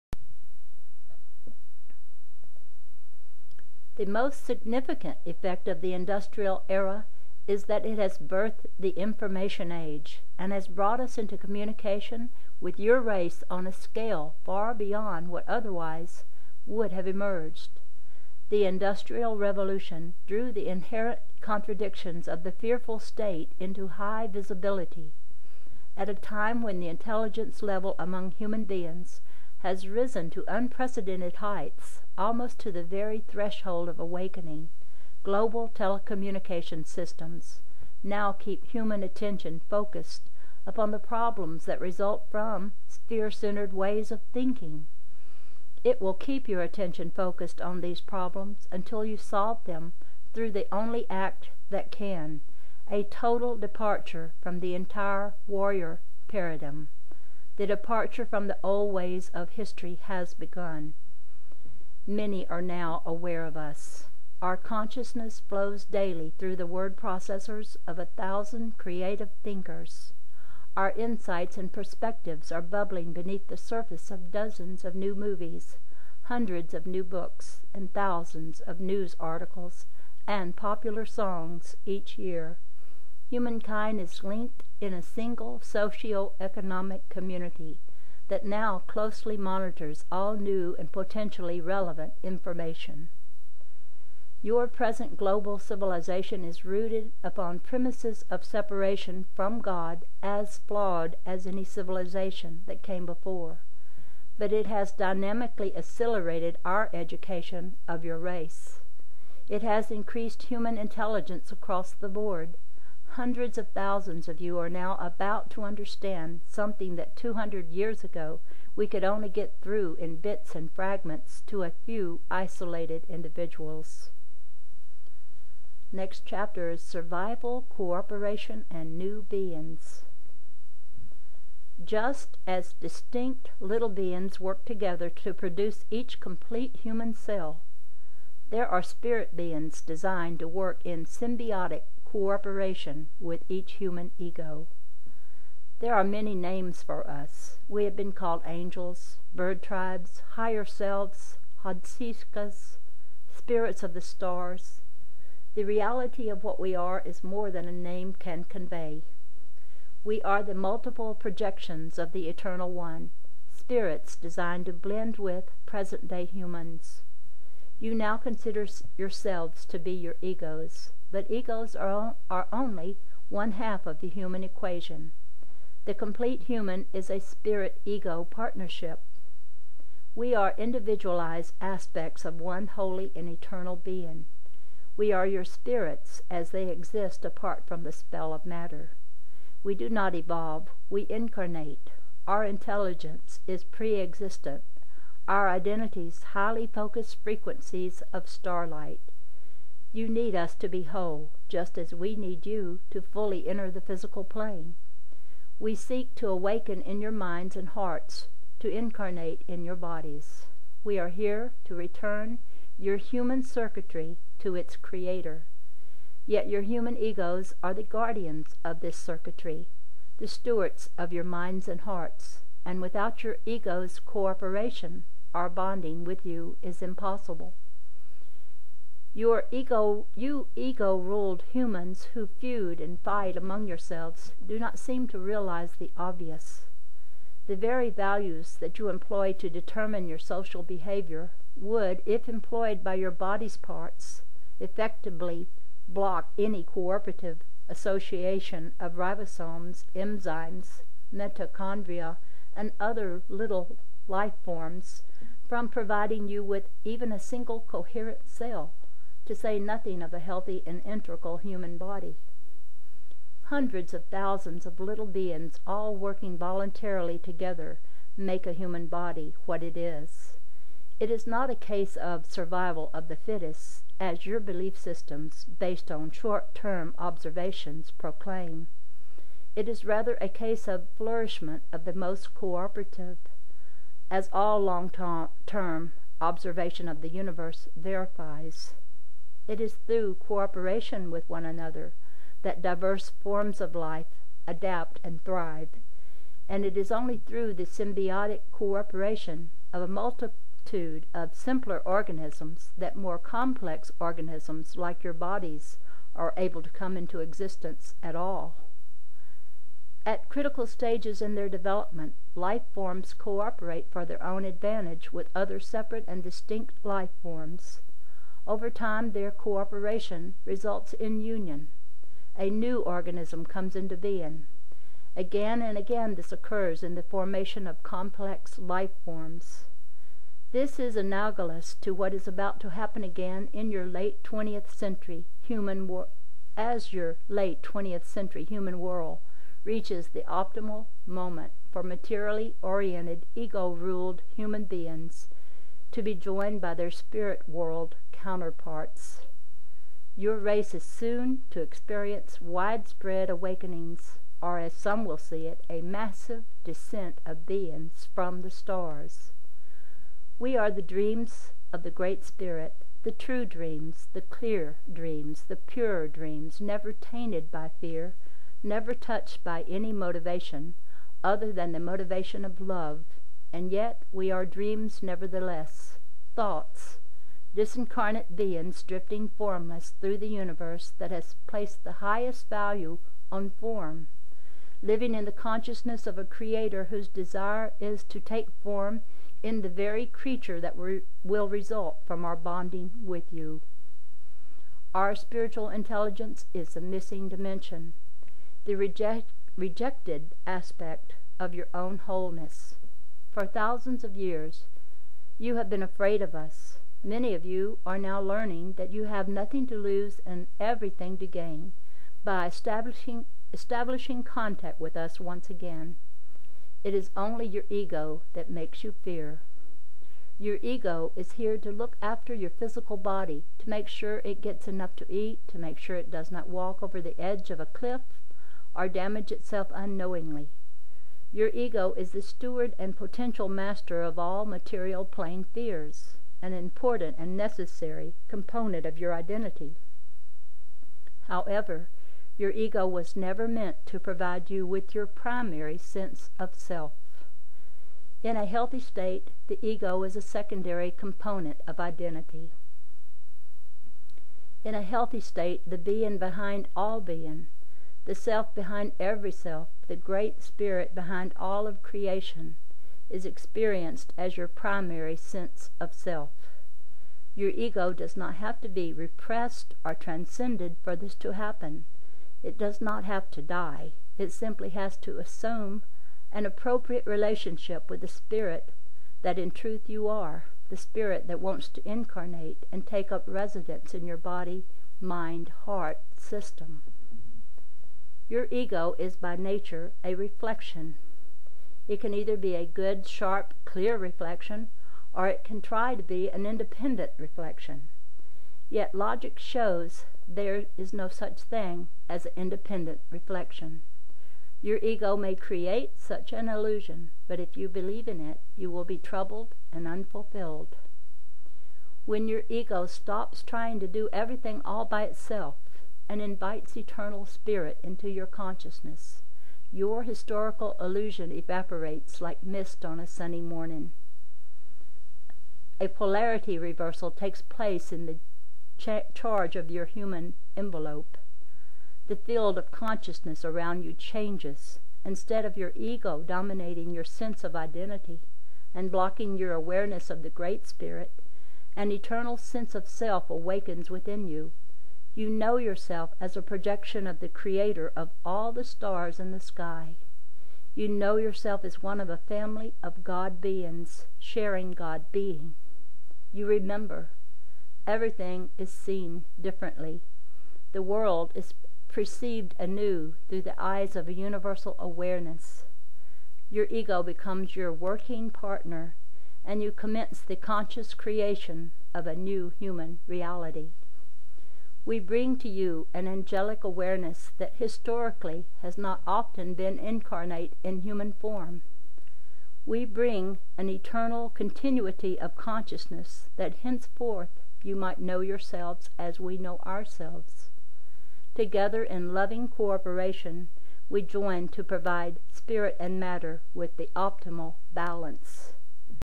09 The Return of the Bird Tribes
Reading Ken Carey's book entitled "The Return of the Bird Tribes"